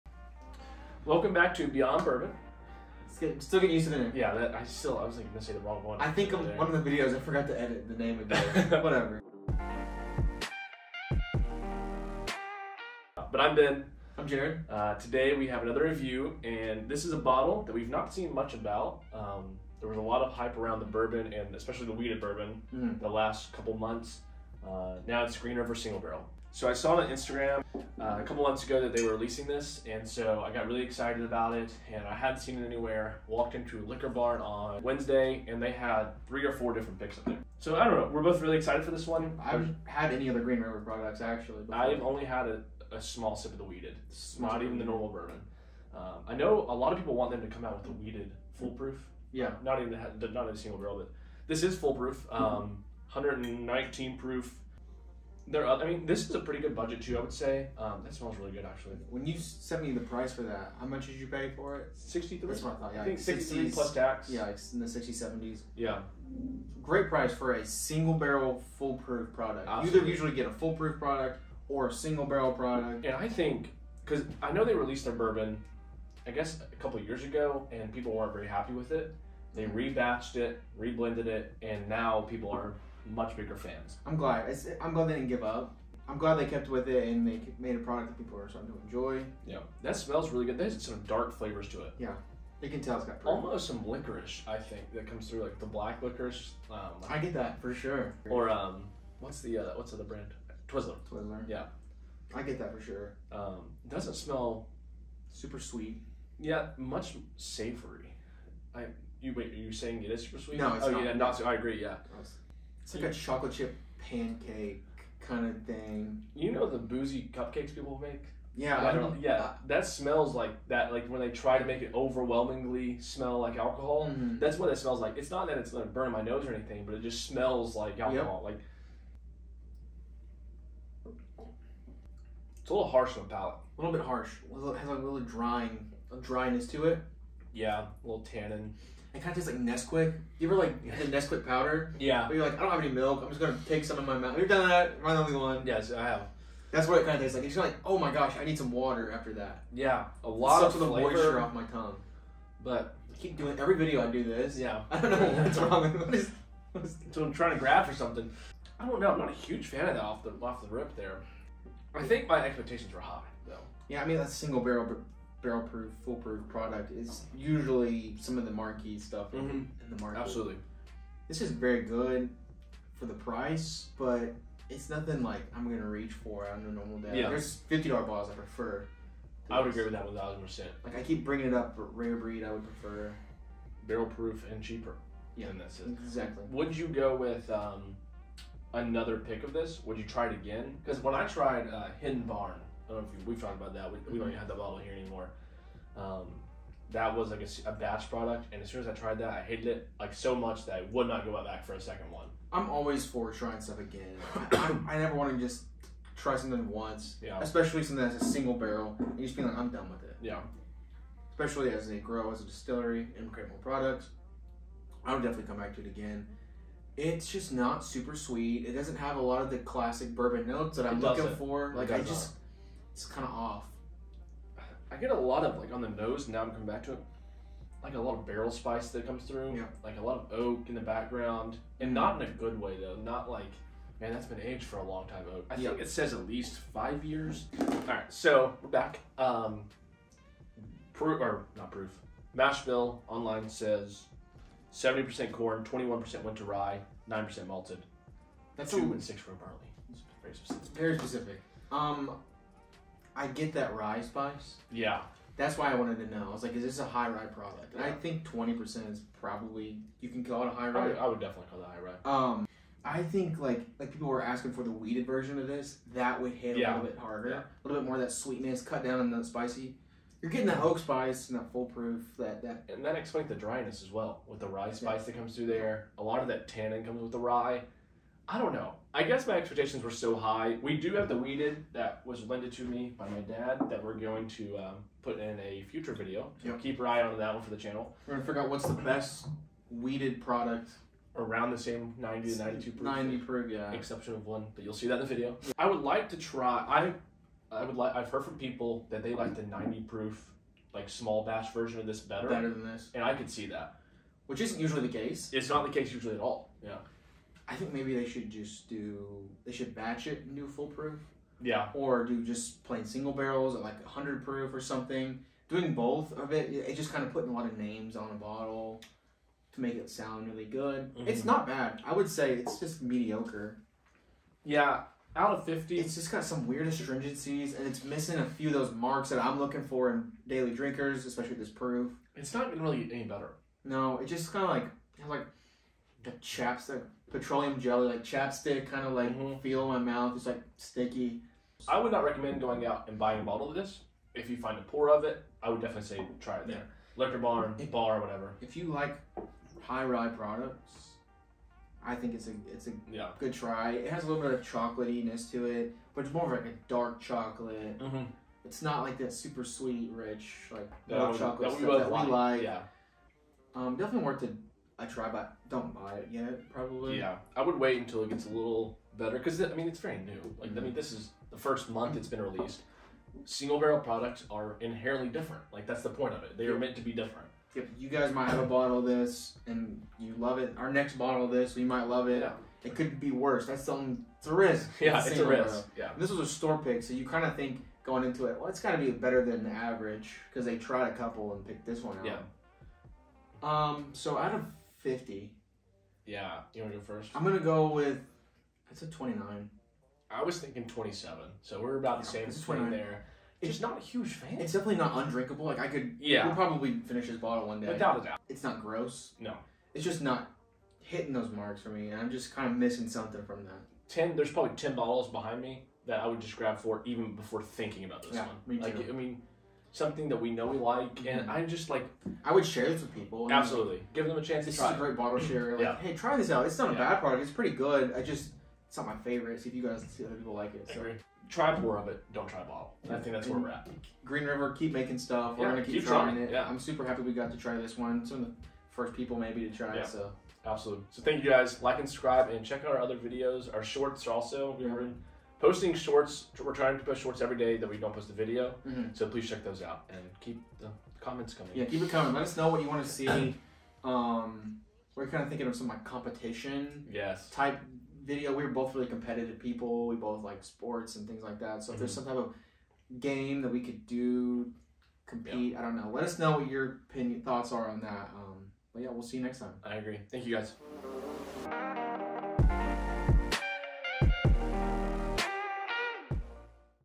Green River Single Barrel Full Proof Review (2023)
Today we taste, discuss, and review the all-new Green River Single Barrel  Full Proof. This bottle comes from a newer distillery, Green River. We were very excited to try the latest offering from Green River after what we have heard about their other offerings. Unfortunately, this bottle did not quite blow our socks off, but for $60 we can't be too upset.

We apologize for the microphones not working.